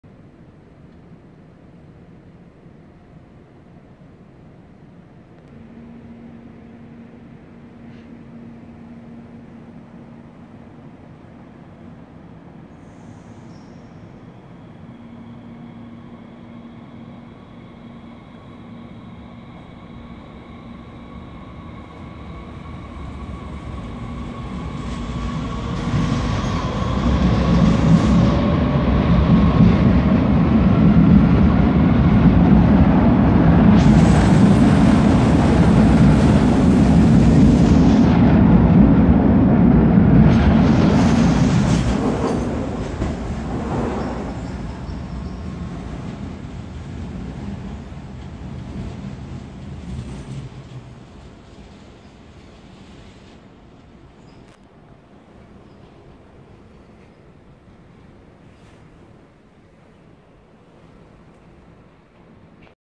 Rhein-Main S-Bahn in Frankfurt(Main)Hbf: S8 Hanau Hbf
Von/from Wiesbaden Hbf(00:49) nach/to Hanau Hbf(02:23)
Am/on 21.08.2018   Mit 10 Min Verspätung